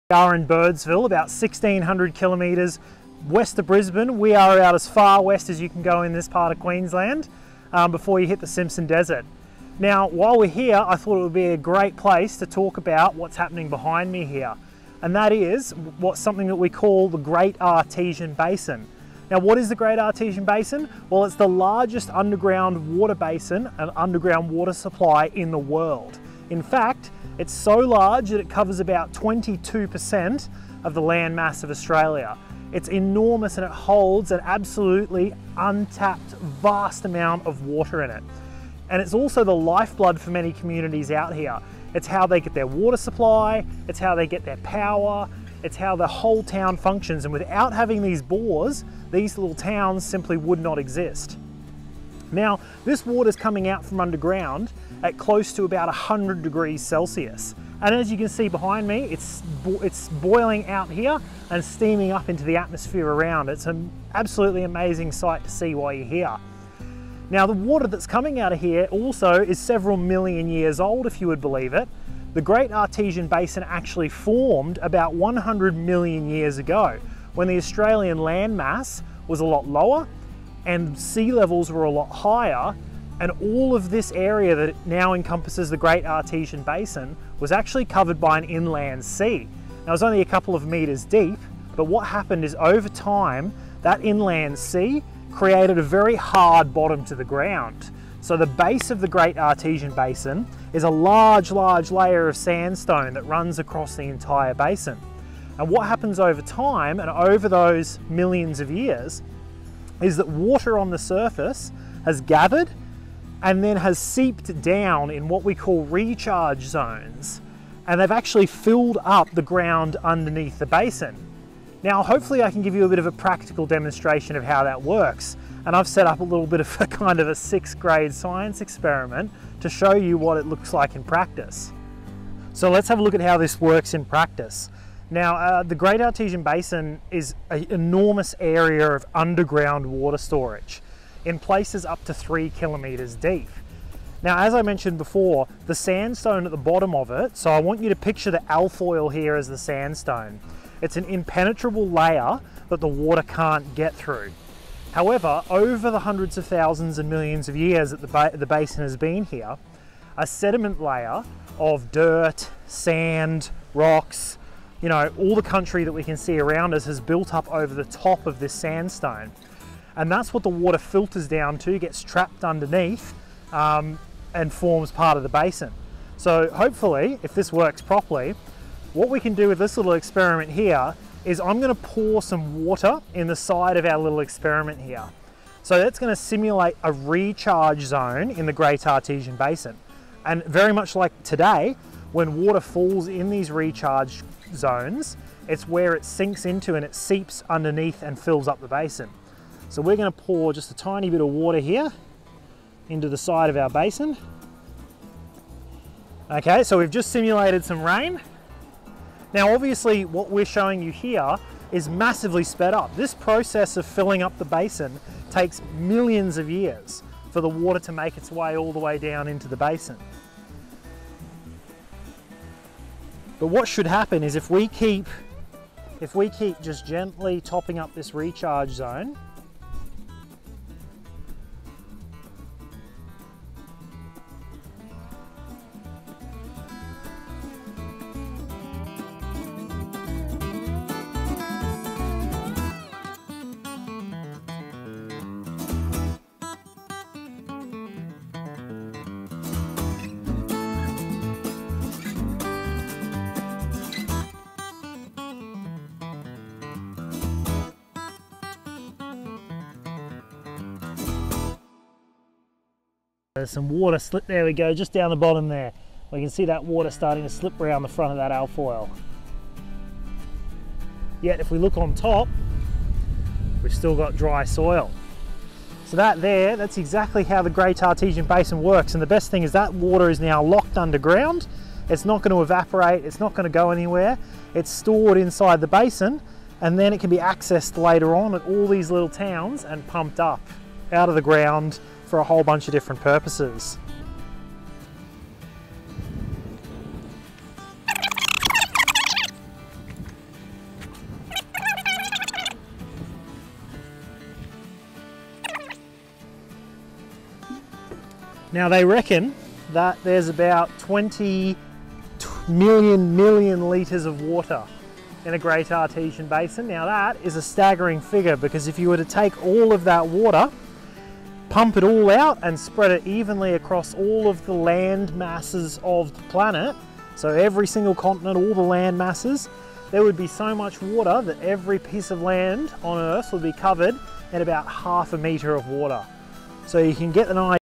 What is the Great Artesian Basin?
The Great Artesian Basin, located in Australia, is the largest and deepest artesian basin in the world. It stretches over 1,700,000 square kilometres and with measured water temperatures ranging from 30–100 °C. The basin provides the only source of fresh water through much of inland Australia supporting hundreds of rural towns and thousands of properties.

The Basin underlies 22% of the continent, including the states and territories of Queensland (most of), the Northern Territory (the south-east corner of), South Australia (the north-east part of), and New South Wales (northern part of). The basin is 3,000 metres (9,800 ft) deep in places and is estimated to contain 64,900 cubic kilometres (15,600 cu mi) of groundwater.